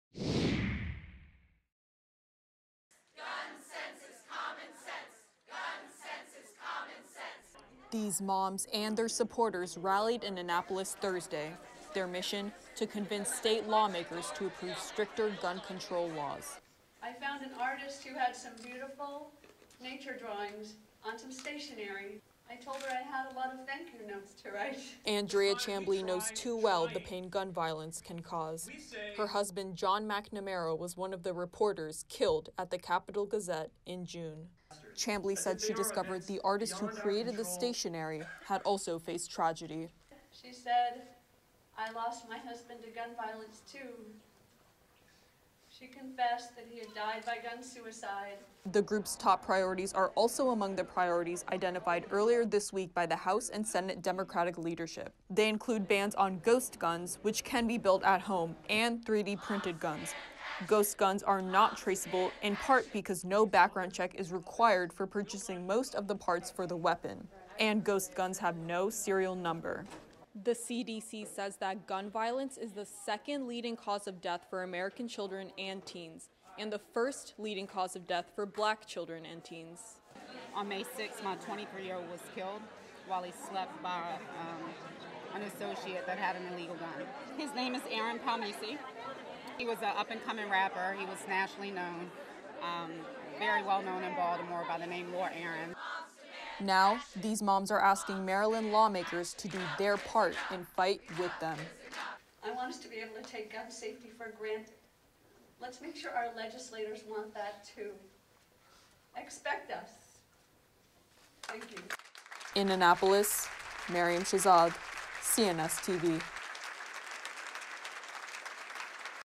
GUN LEGISLATION: Mothers take on State House to push for additional gun control
ANNAPOLIS, Maryland – They call themselves “Moms Demand Action for Gun Sense in America” and members of the Maryland chapter came to Annapolis on Thursday to push for new gun control provisions. The moms and their supporters say they want the legislature to ban “ghost guns” and 3D printed guns as well as require background checks for anyone wanting to buy a shotgun or long guns.